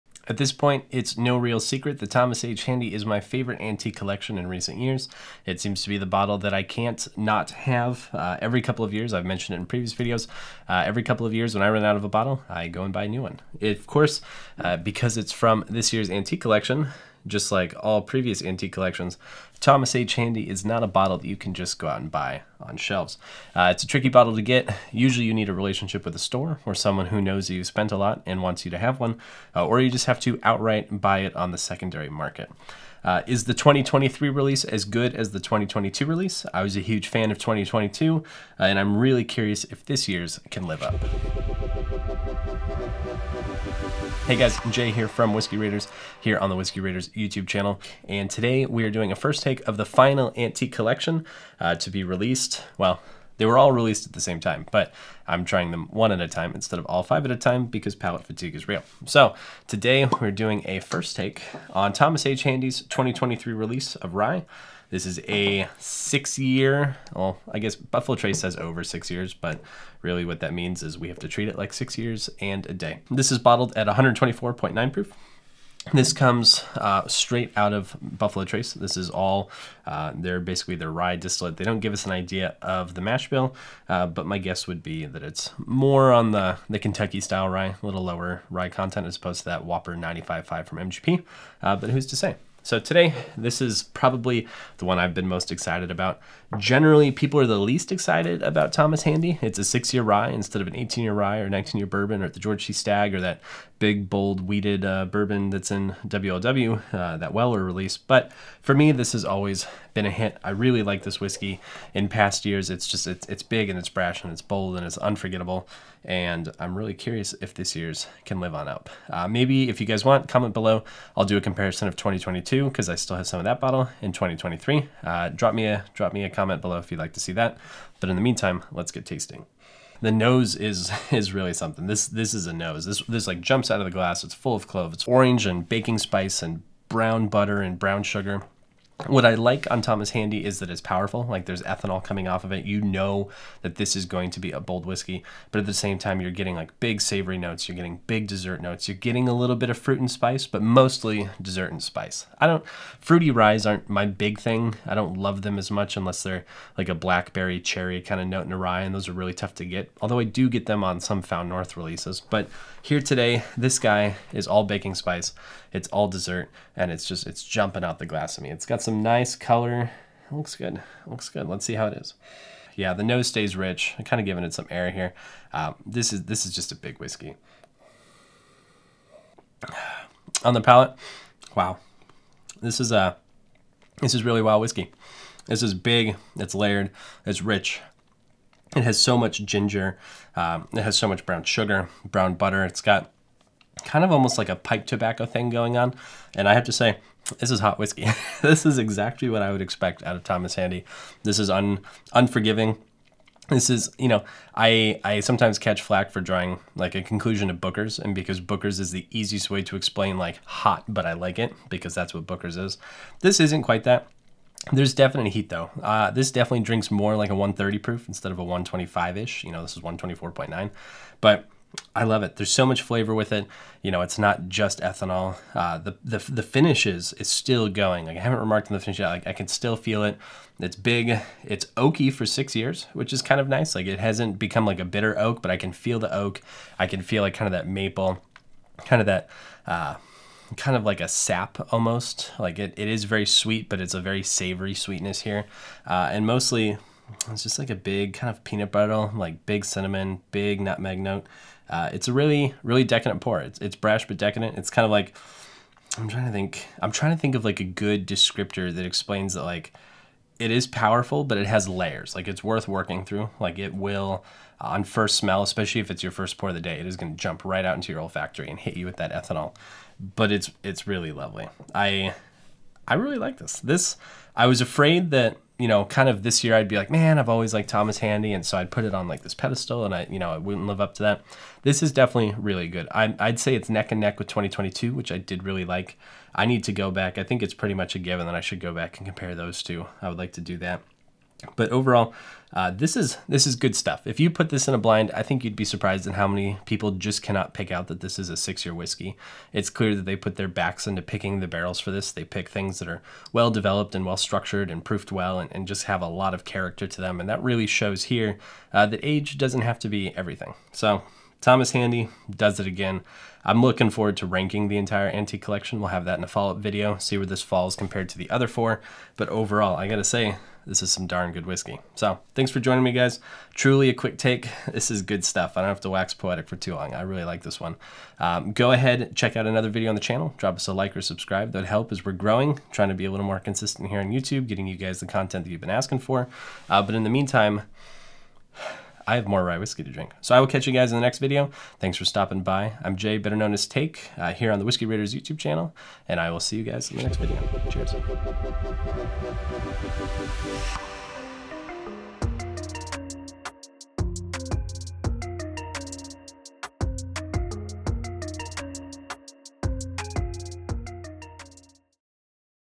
Thomas H Handy 2023 Rye Whiskey Review (Buffalo Trace Antique Collection) | First T8ke
Today's First T8ke is the first taste impressions and breakdown of Thomas H Handy Sazerac Rye from the 2023 Buffalo Trace Antique Collection release. This is a Kentucky Straight Rye Whiskey bottled at barrel proof. Let's dive in!